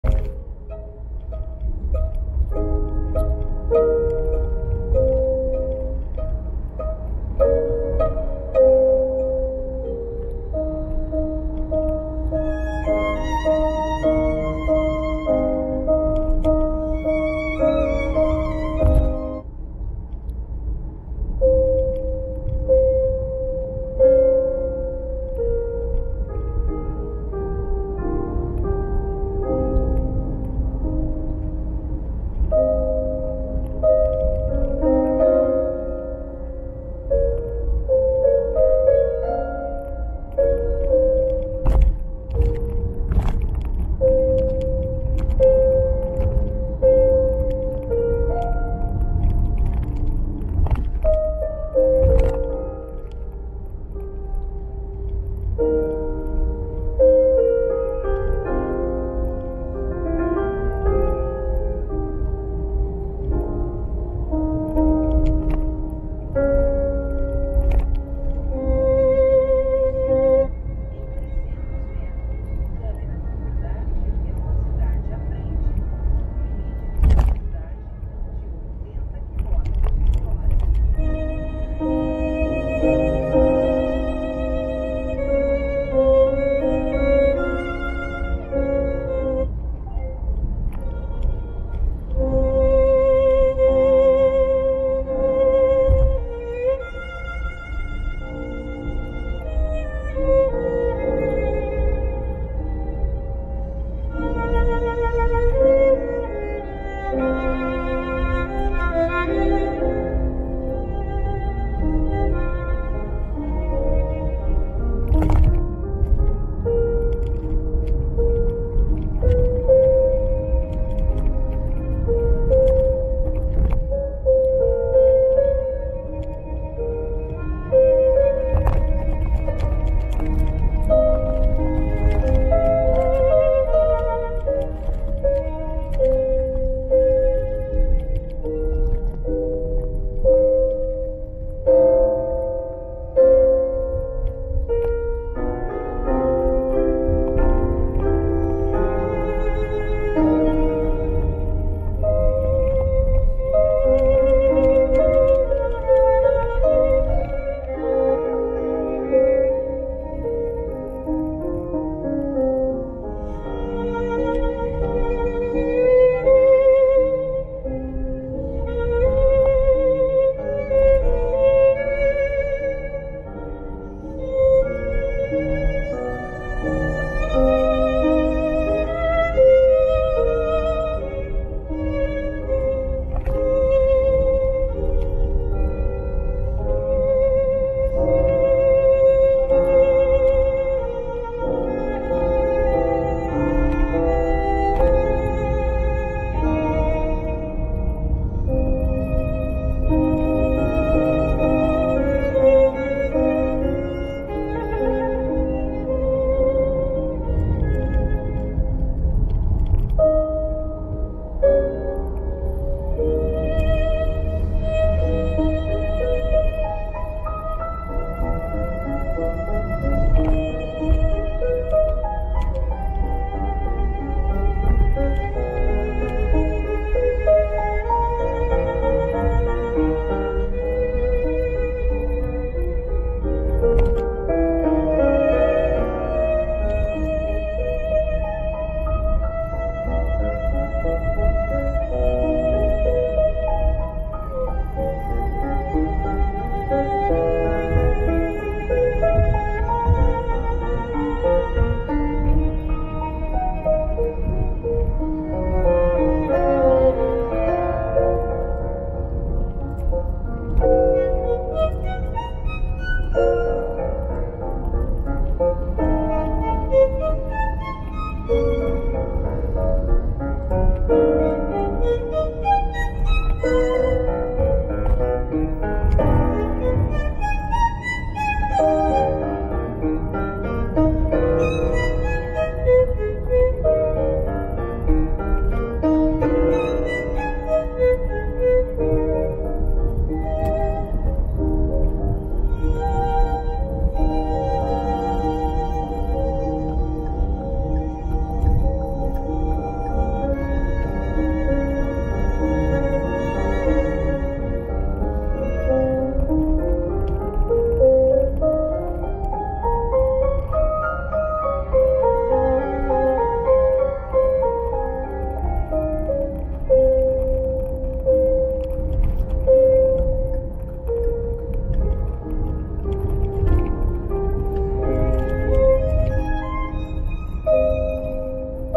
BR101, PONTE RIO-NITEROI, PARTE 1 DJI 0209
Viagem de volta do plantão no Rio, uma segunda feira, 2 de novembro de 2020, dia nublado.  Com música, filmado em 4k, 30 FPS, na Osmo-Pocket.
Aqui, Beethoven, sonata para violino e piano.